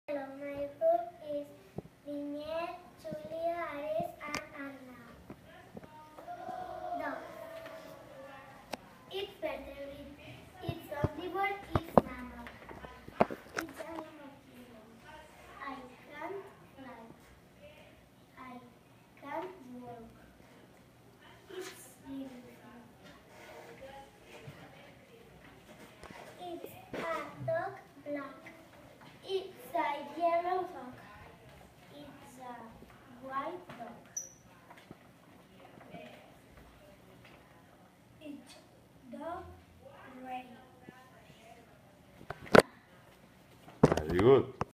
Animals - 2nd Primary - Sant Ramon de Penyafort (1)
The 2nd of primary students analyzed the characteristics of an animal, we created a mural and we did an oral presentation of it.
The best thing is that all the process was made by themselves, they searched the vocabulary in the computers and tablets and they planned the presentation based on a basic structure that they know.
Finally, we corrected the spelling mistakes and the grammar.